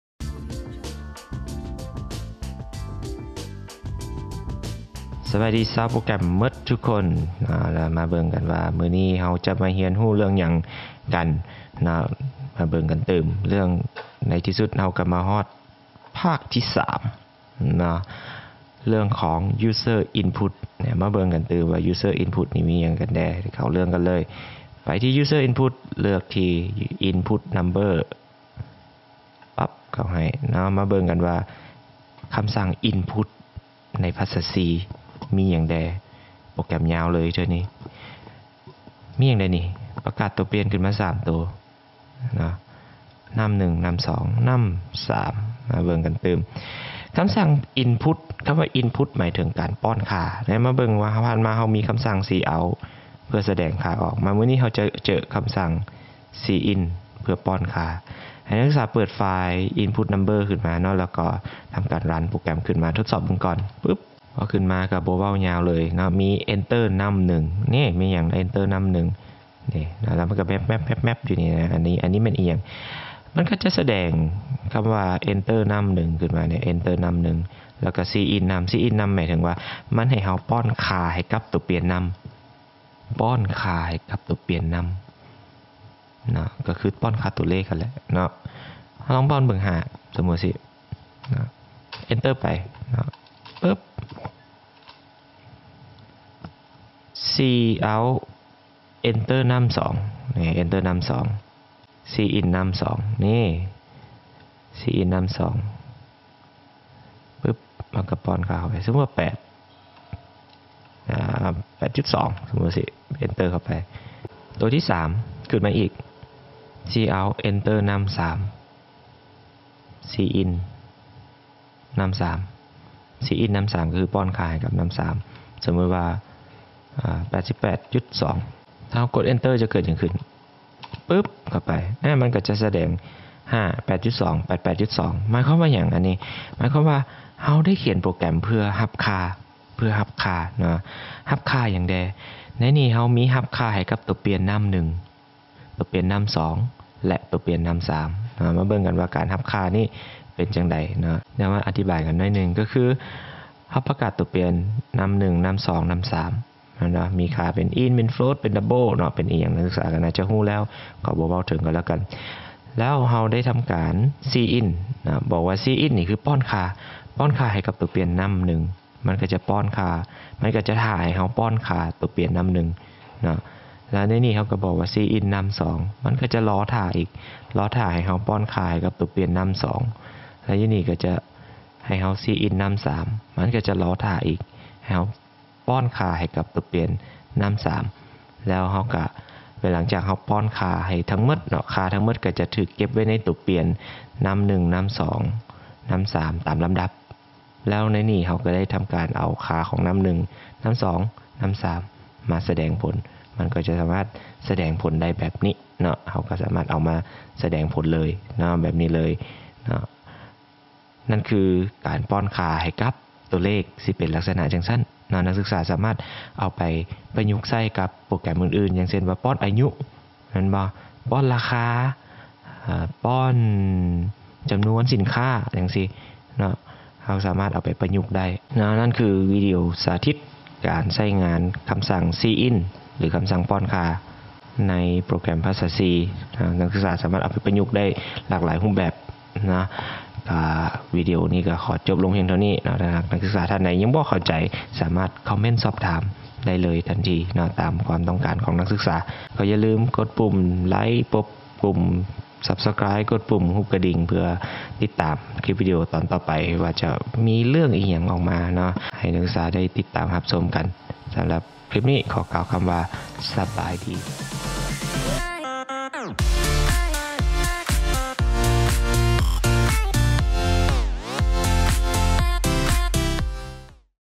C++ Programming : ການປ້ອນຄ່າໂຕເລກ
ວິດີໂອສາທິດການປ້ອນຄ່າ ຫຼື ການຮັບຄ່າໂຕເລກດ້ວຍຄຳສັ່ງ cin ໃນພາສາ C++
c++, ຂຽນໂປຣແກຣມ, ພາສາ C++, dev-c++, ວິທີຂຽນໂປຣແກຣມ, yaklao, haamlezz,